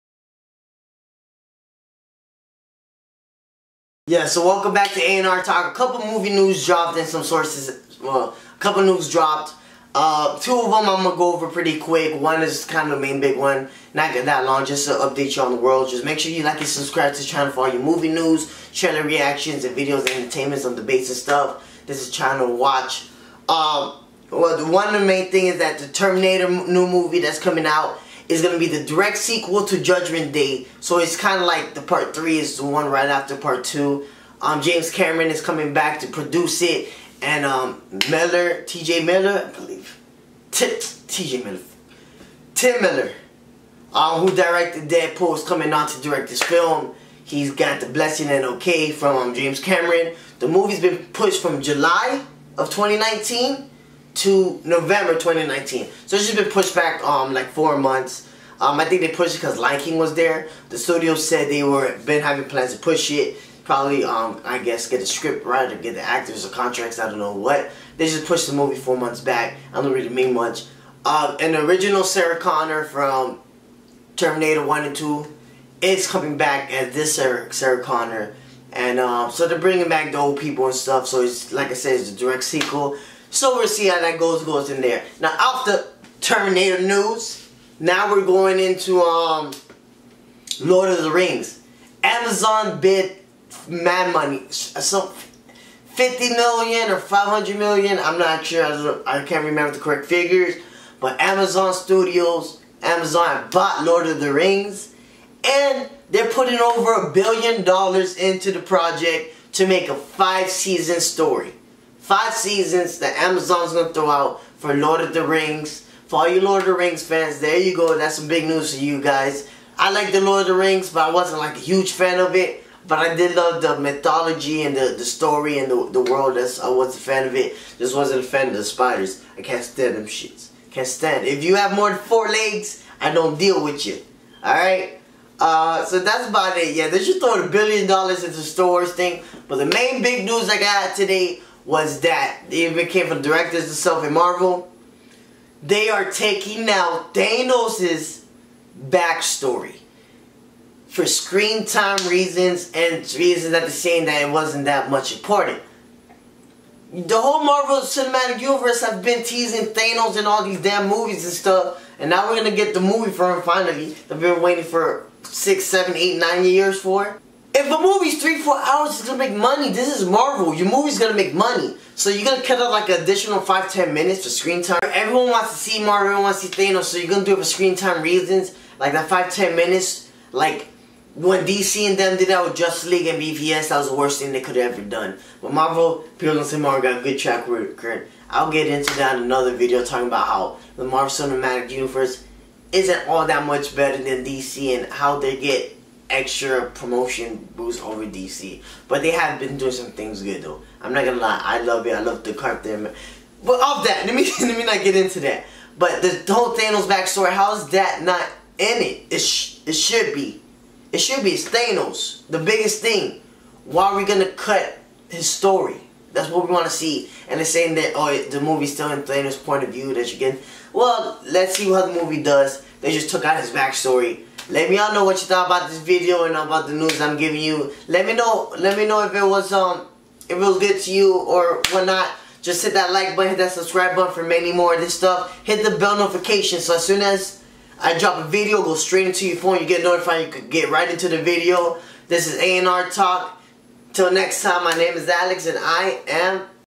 Thanos back story cut in Infinity war..Terminator gets pushed back and LOTR Getting a TV SHOW!!
Movie News..I discuss the newest topics that came out on the movie and TV world. Thanos backstory cut in avengers. Terminator movie being pushed back and a LOTR TV show by Amazon.